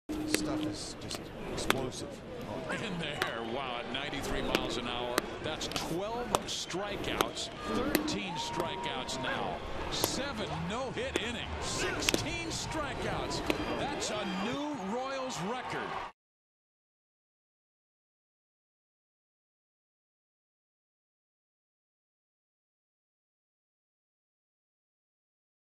Duffy's 16 strikeouts in 16 seconds
Watch all 16 of Danny Duffy's strikeouts vs. the Tampa Bay Rays in 16 seconds